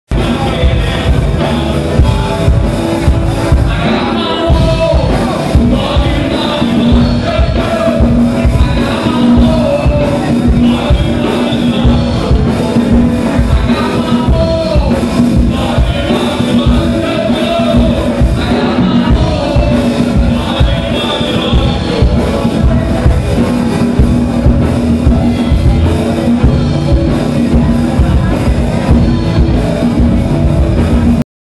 DantasDesigner_CNEC_20novembro2009
Festa de formatura dos alunos da CNEC, entrega de diplomas e homenagens para pessoas especiais. Parabéns formandos 2009. Filmagem e fotos: DantasDesigner.